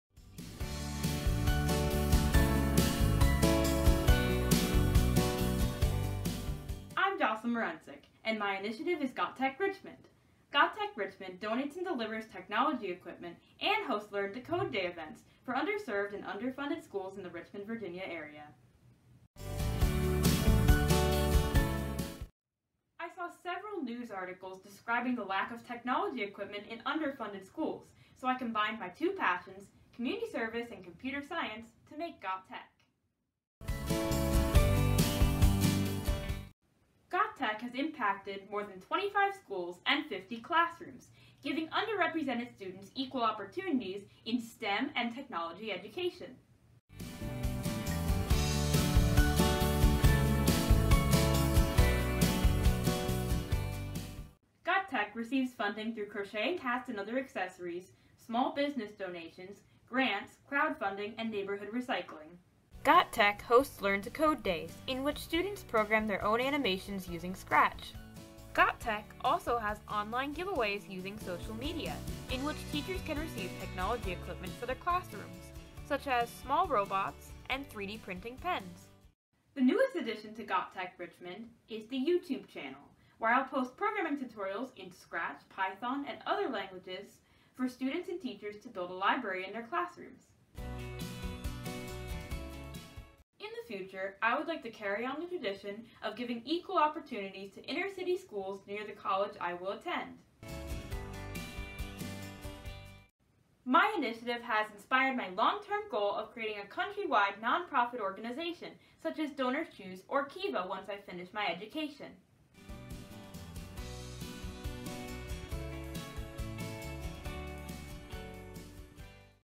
Got Tec! Richmond - Trailer
Got Tec! Richmond is a community initiative that donates and funds technology equipment, and teaches "Learn-to-Code" classes for under-served and under-funded schools in the Richmond, Virginia area.